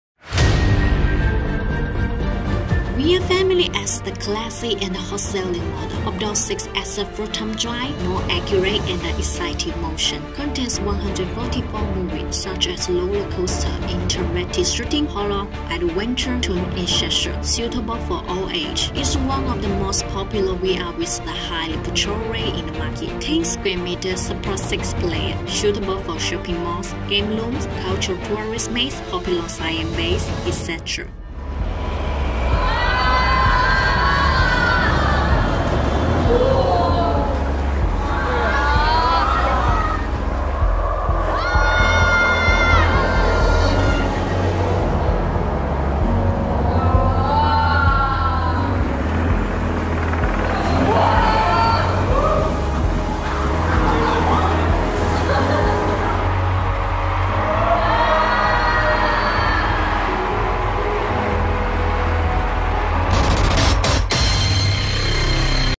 Hot Selling VR Family simulator 6 seats 9D cinema popular project in shopping mall
VR Family simulator is a super hot simulator in market, especially in shopping mall, events, parties, etc.
It allow 6 people to play together, no matter excited roller coaster ride or HD gun shooting game, VR family can meet all consumer group's demand.